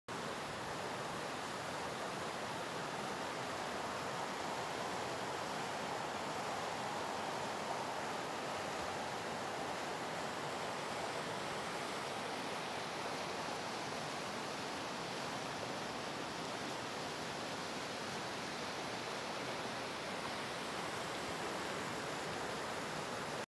Christmas by the Magnetawan River, Britt Ontario ... peaceful
Quick stop to watch the ever churning waters of the Magnetawan River by Britt Ontario